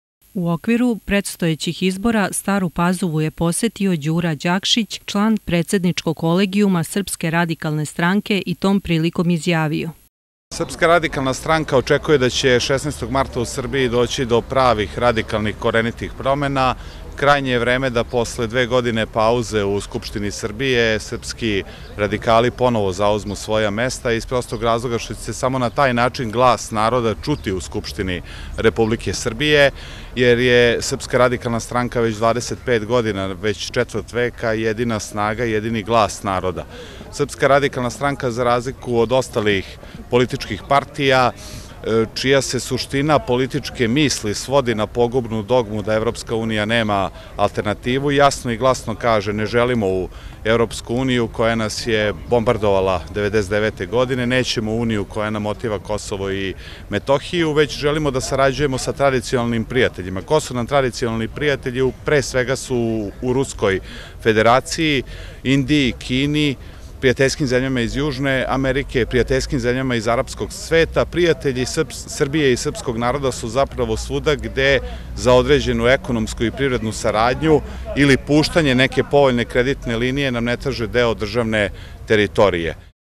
Izbori 2014 Srpska radikalna stranka
Đura Đakšić član kolegijuma SRS boravio danas u Staroj Pazovi.